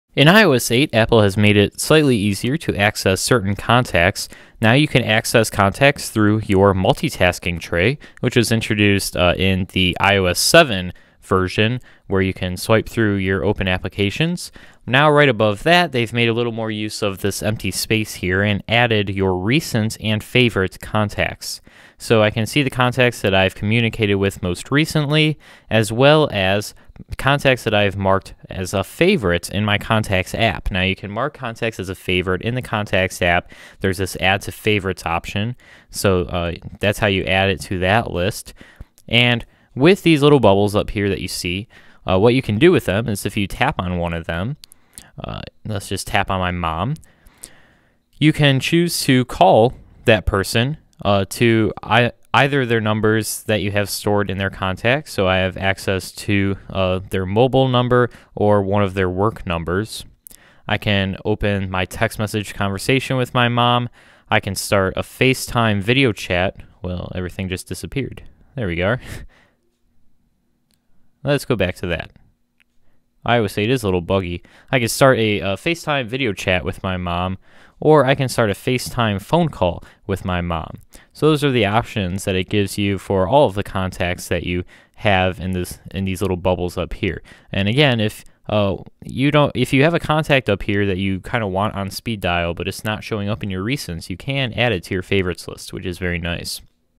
Quick Contact Access from Multitasking in iOS 8
In iOS 8, Favorite and Recent contacts can be accessed quickly from the Multitasking Tray by double tapping the home button.

iOS 8 will be available to the public Fall 2014.